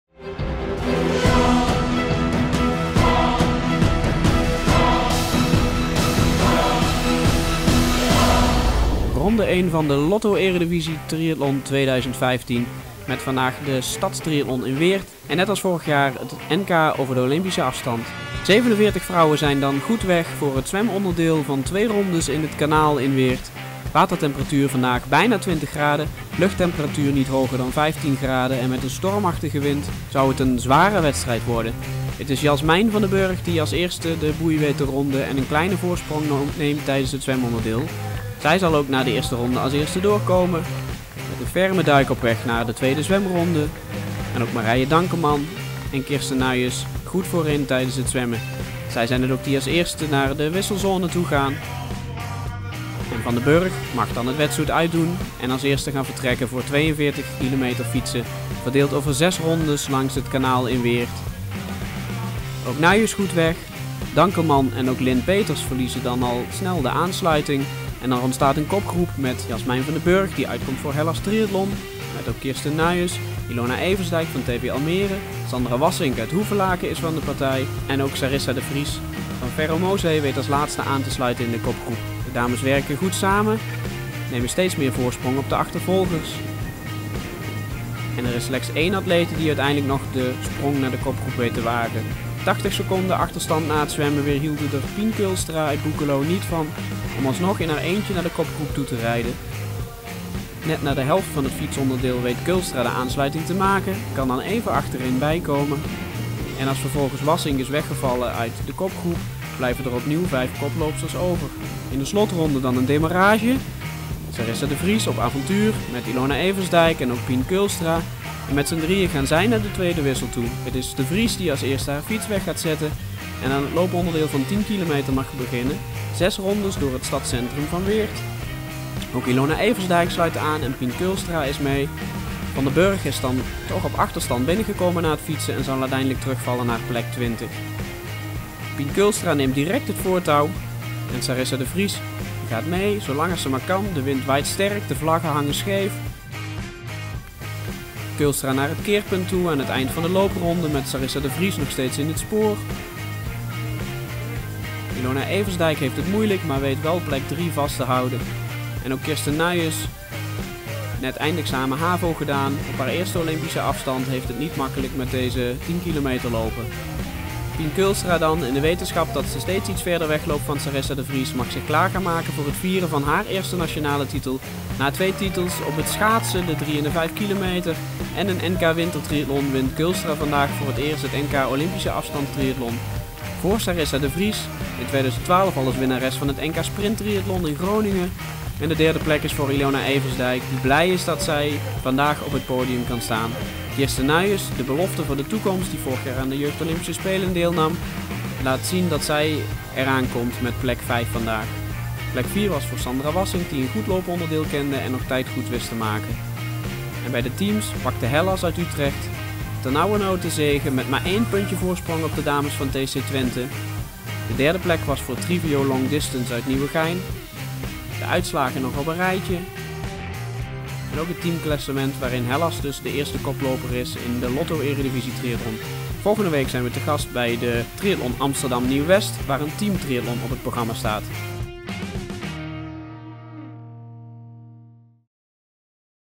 Eredivisie Triathlon - Weert NK Olympische Afstand 2015 Vrouwen
Het NK OD was de openingswedstrijd van de Lotto Eredivisie Triathlon 2015. Pien Keulstra won haar eerste titel. Hellas Utrecht was het snelste team.

Top 3 NK:
1. Pien Keulstra
2. Sarissa de Vries
3. Ilona Eversdijk

Top 3 teams:
1. Hellas - Utrecht
2. TC Twente - Enschede
3. Trivio/Long Distance Team - Nieuwegein